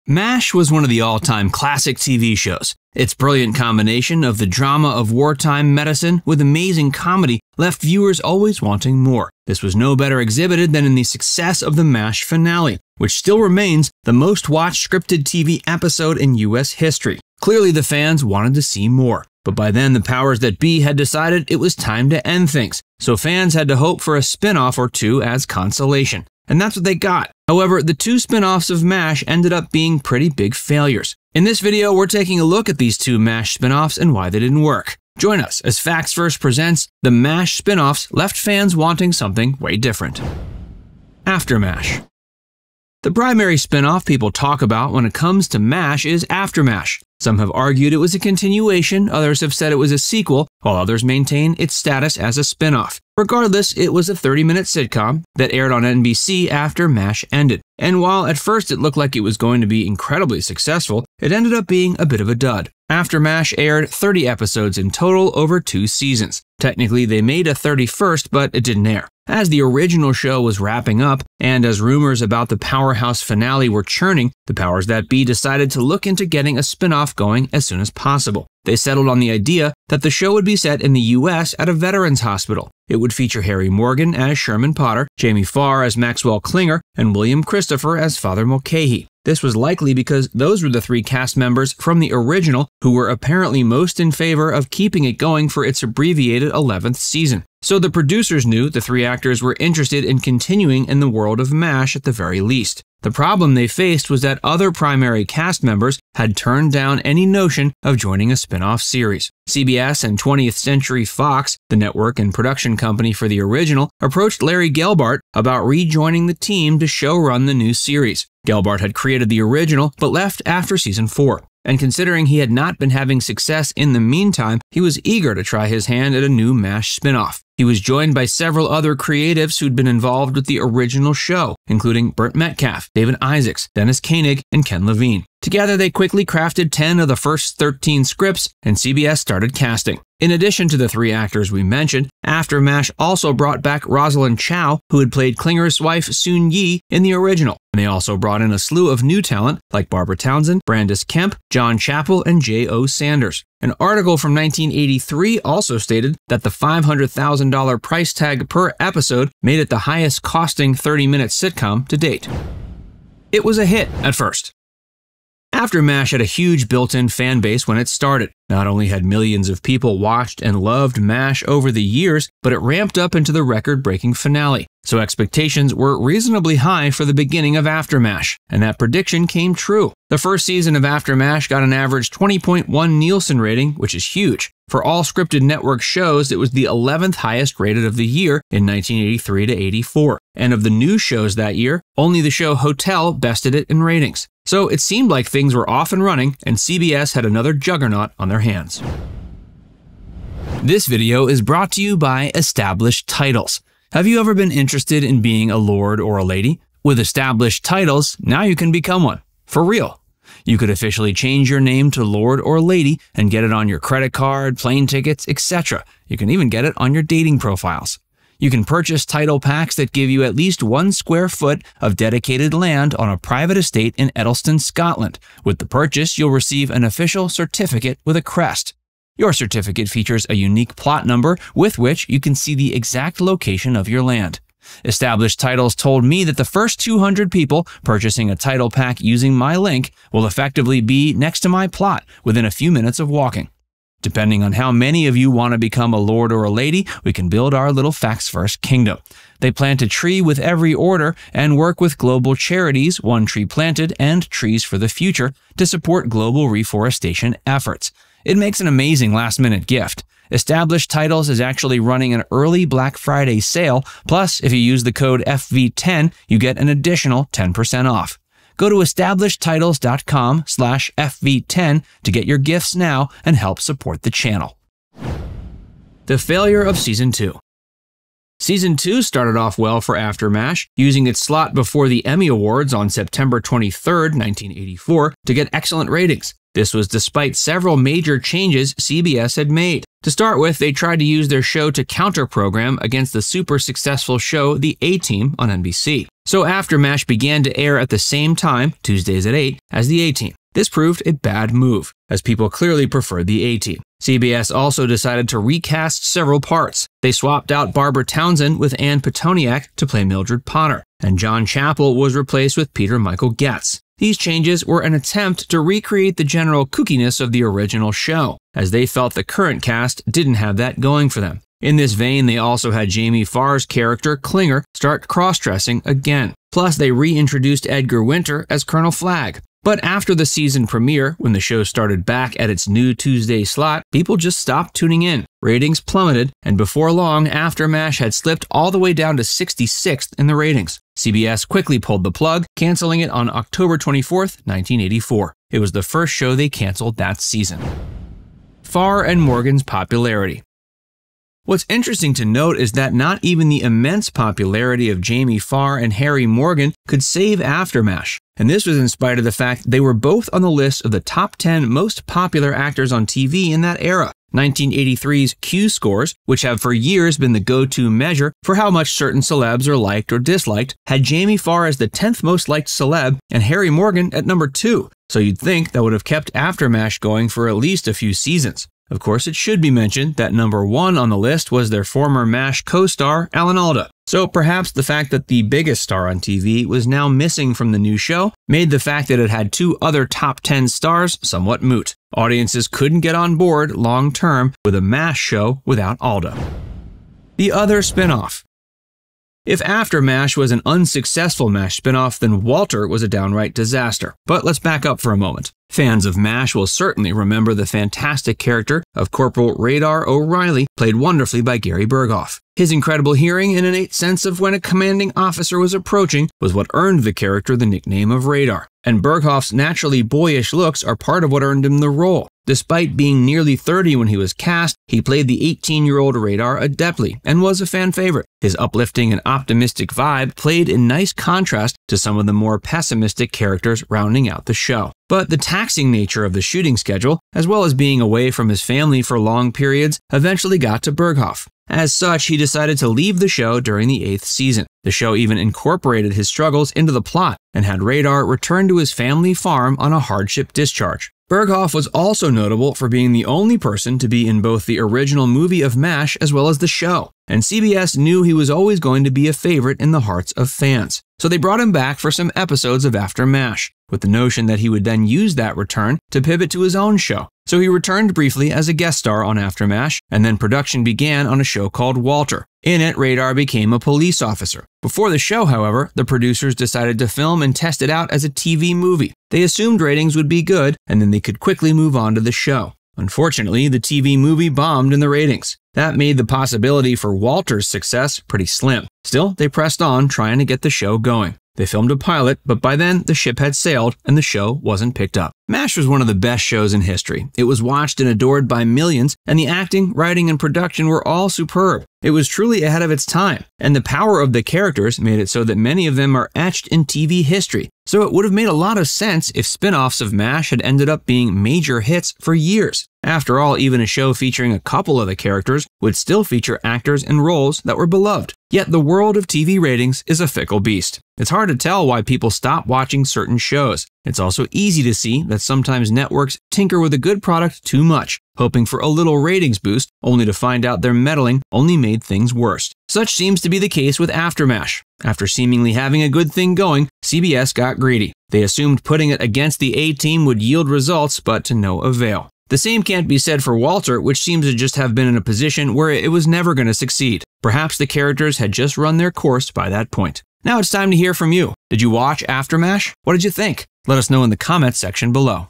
The M*A*S*H Spin-Offs Left Fans Wanting Something Way Different
MASH is considered one of the best shows to ever be on the air. It perfectly blended comedy and drama in a way that resonated to viewers for years. And it still stands the test of time as one of the best shows to ever be on TV. So you'd think that such a powerful and popular show would have at least a few successful spin-offs. After all, when you think of the shows that have yielded several great spin-offs, most of those originals were less popular than MASH. And yet, when, after the 11th and final season of MASH aired, and a spin-off was created, it ended up being a failure. Not at first however.

00:00 - Intro
00:41 - AfterMASH
02:43 - It Was a Hit… at First
04:36 - The Failure of Season 2
05:48 - Farr and Morgan’s Popularity
06:37 - The Other Spin-off
09:10 - Outro

When AfterMASH began, it picked up where MASH had left off, ratings-wise. And while it didn’t bring back a lot of the primary cast members, it had three big names (Harry Morgan as Sherman Potter, Jamie Farr as Maxwell Klinger, and William Christopher as Father Mulcahy) from the original. And at first, it was a hit. It ended its first season as the second highest rated new how of the year. Yet the network got hubristic and tried to push the limits of the show. They not only swapped out cast members to get it to feel more like the original show, but they also decided to pit it against the biggest show at the time, The A-Team. This resulted in disaster.

AfterMASH never recovered after plummeting in the ratings and was canceled by CBS before the second season even finished. In this video, we’re taking a closer look at what happened to AfterMASH, as well as the other complete failure of a spin-off, called WALTER. So stick around, as Facts Verse presents: The M*A*S*H Spin-Offs Left Fans Wanting Something Way Different.